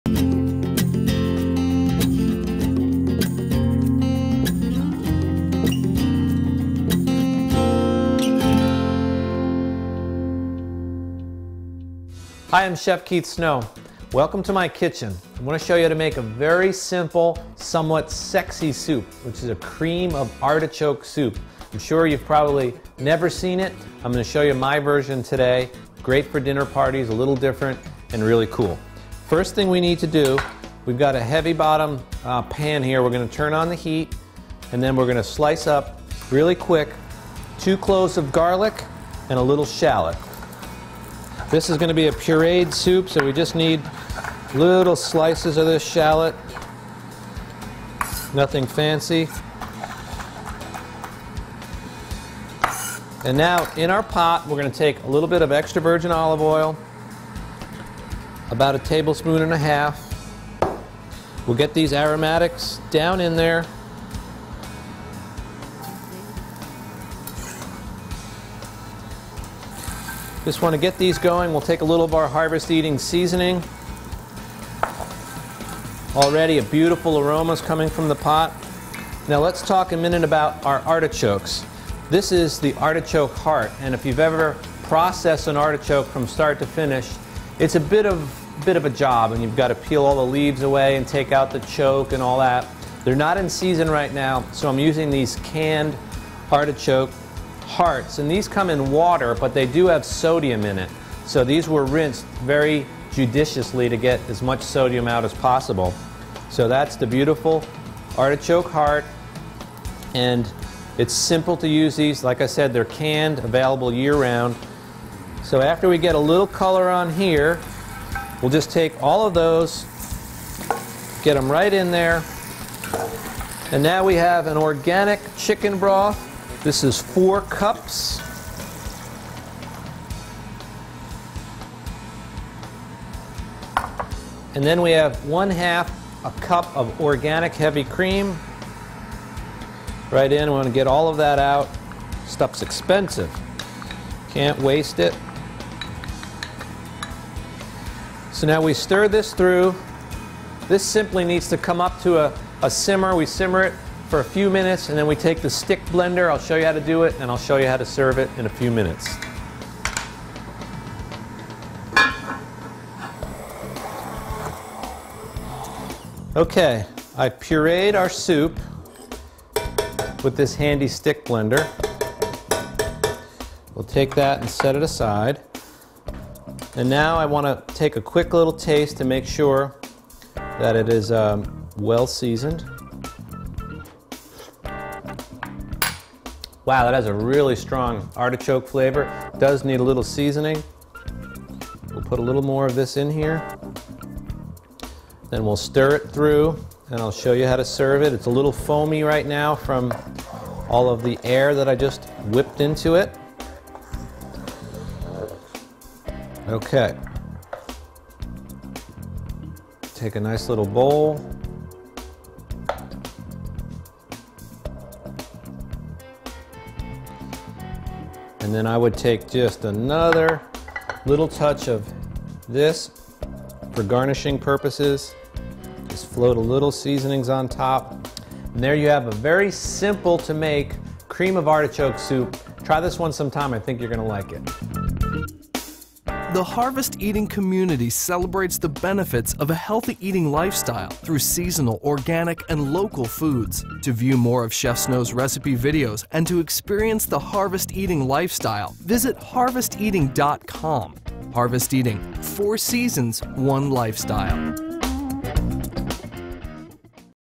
Video Recipe: Cream of Artichoke Soup
to get the written recipe for this video. This artichoke soup is easy to make, very satisfying and nice to serve at dinner parties.